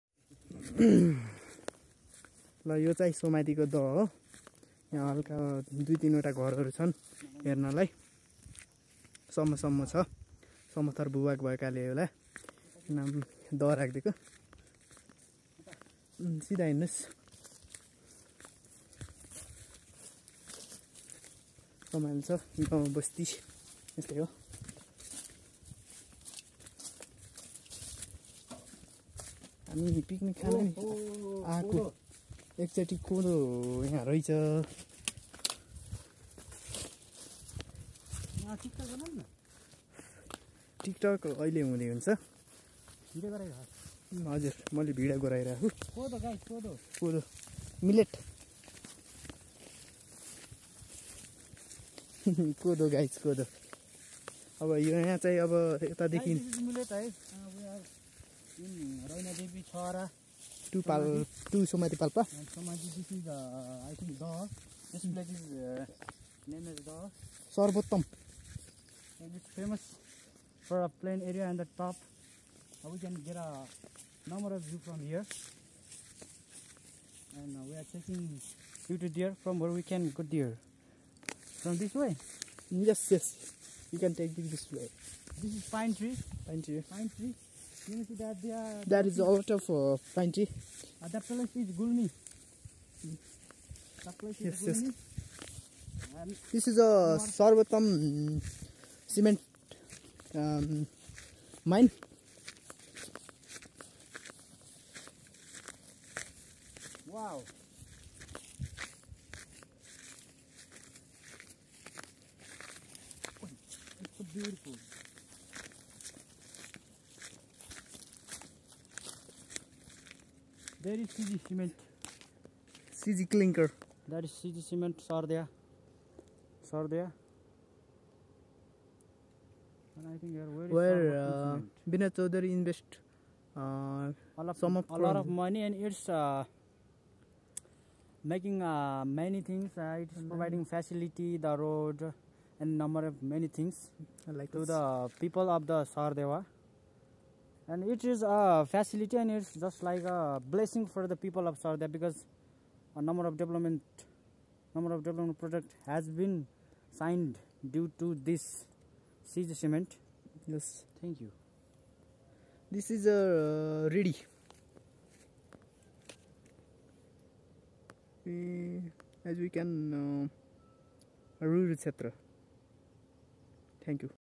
A visit to The Palpa.somadi दह
A short travel to रैनादेवी छहरा-२ सोमादी दह ।from where different types of view can be observe like the himlayan range and the ongoing C.G cement project on Sardewa.